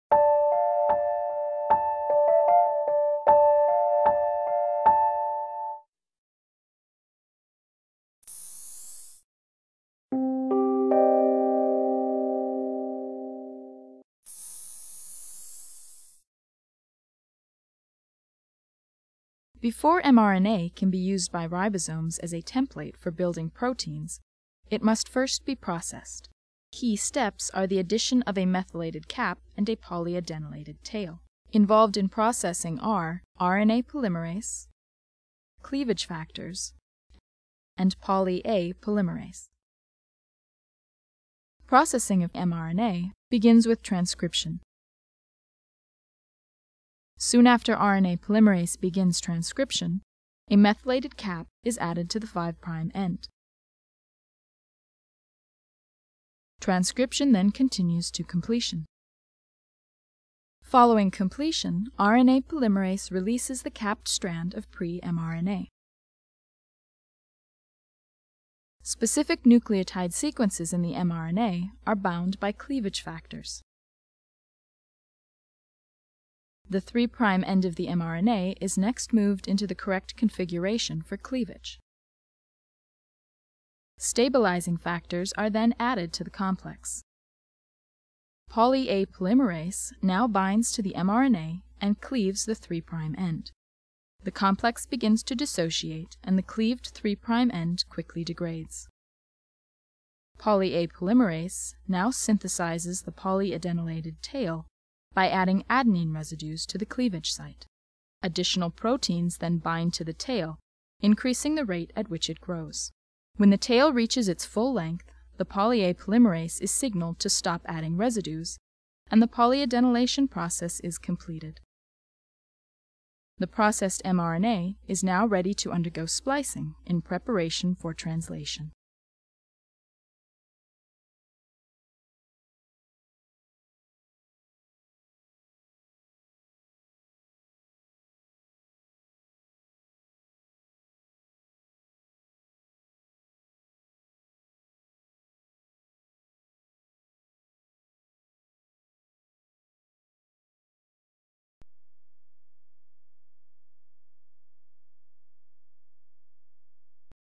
Processamento do mRNA
Depois de transcrito, o pré-mRNA deve ser processado. Durante o processamento, são adicionados certos componentes ao mRNA. Nesta animação podem ver-se os passos básicos do processamento do mRNA. Não fazendo estritamente parte do programa de Biologia do 11o Ano, pode ser usada para dar uma visão mais aprofundada da temática, de uma forma visual e compreensível, ou no âmbito do Ensino Superior.